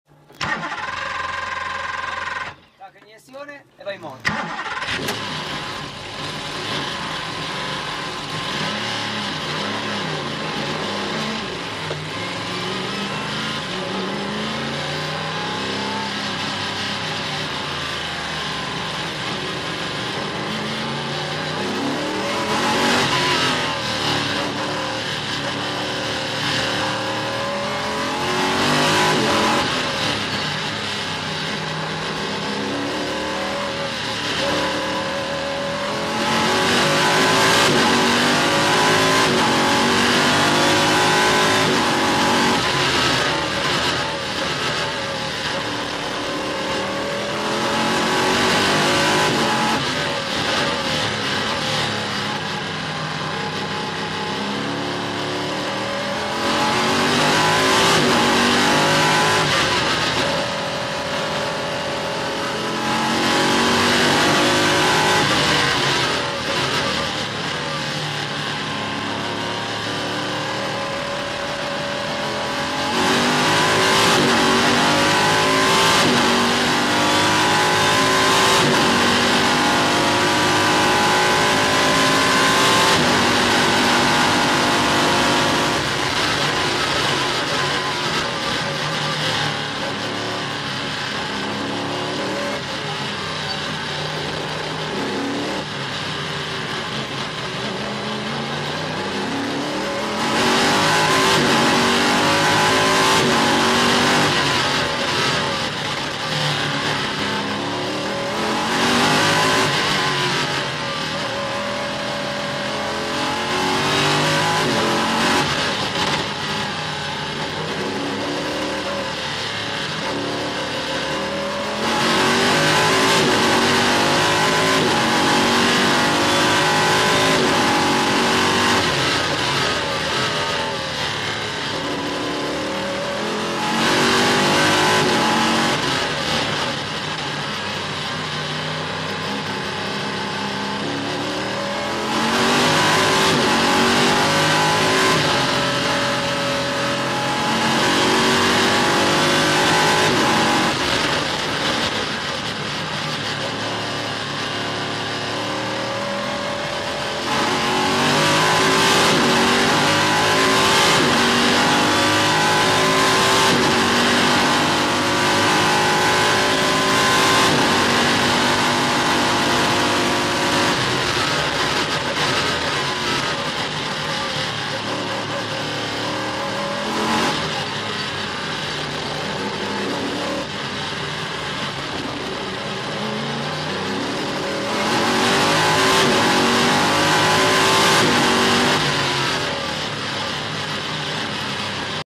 Checco Malavasi Test Ginetta G57 (LMP3) San Martino del Lago September 06 / 2016
Checco Malavasi
Test Ginetta G57 (LMP3)
San Martino del Lago